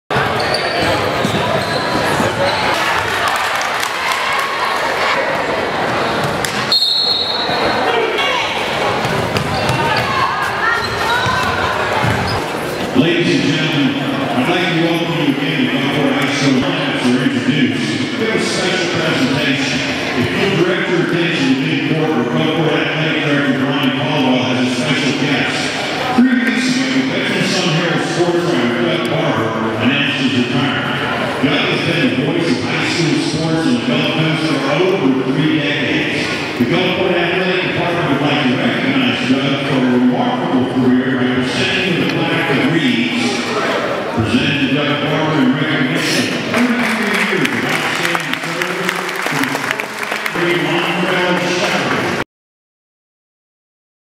Doug Barber honored by Gulfport High
Sun Herald sports writer Doug Barber is honored by Gulfport High for his 33 years of covering prep sports in South Mississippi